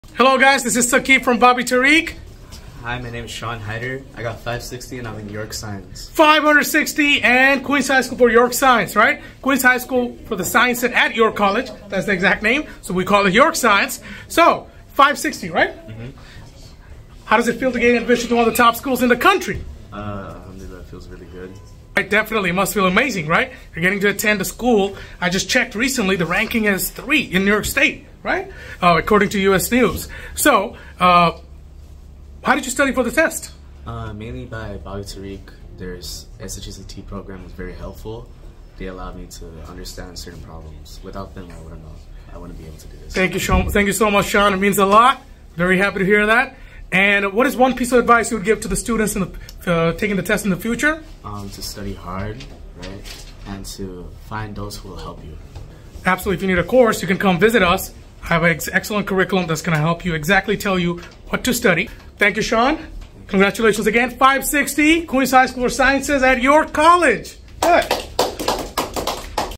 Accepted to Queens High School for the Sciences at York College | Rank 1 U.S.News | Bobby-Tariq
My student, Shaan Haider, scored 560 on the SHSAT and was accepted to York Science. According to U.S.News, Queens High School for the Sciences at York College is ranked first in New York. Congratulations, Shaan.

By: Sakib Hussain
CEO & SAT/SHSAT Instructor